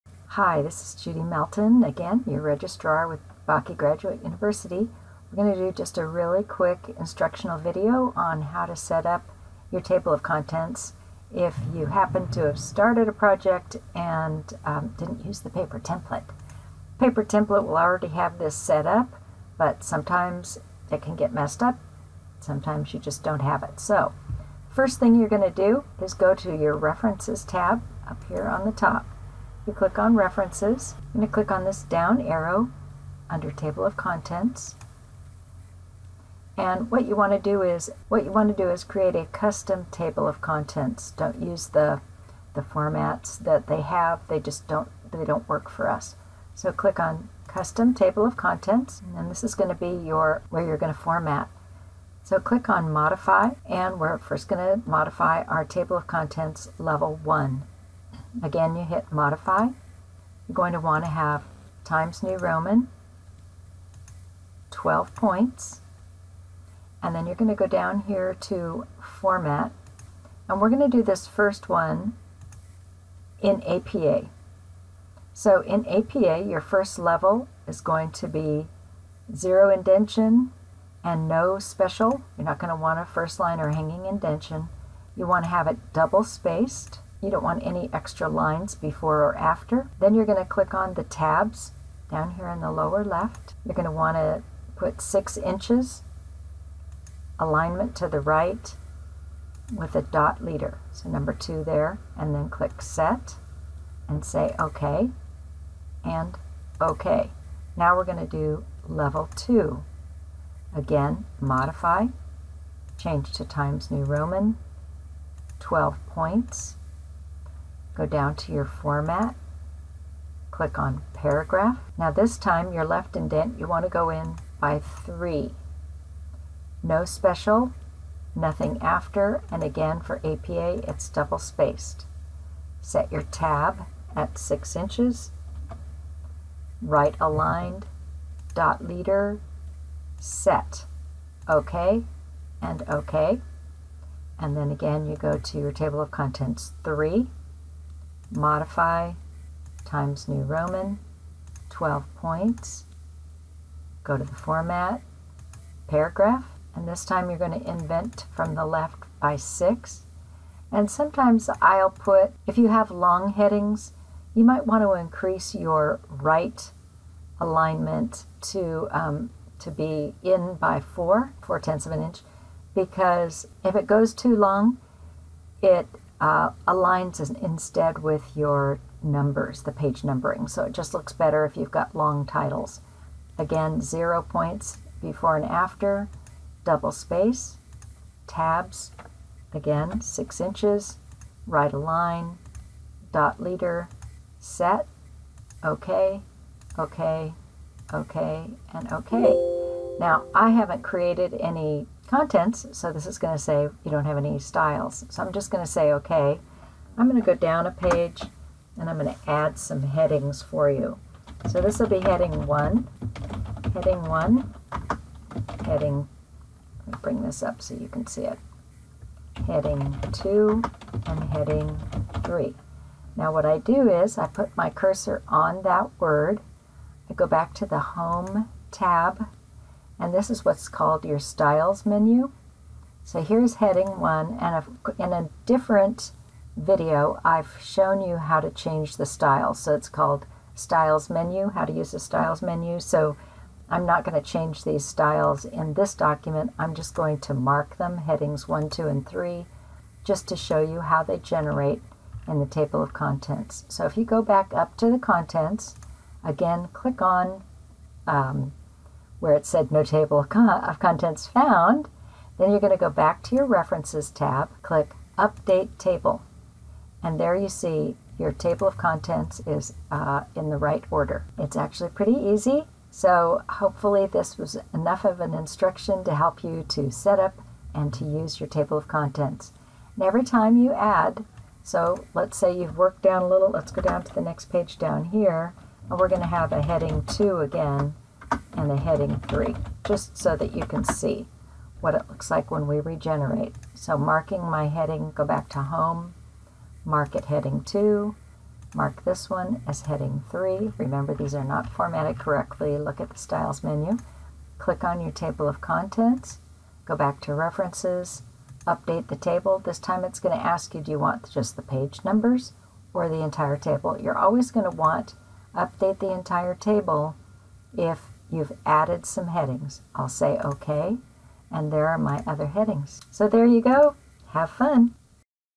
Table of Contents APA
Instructions on creating a table of contents in Word for Bakke Graduate University students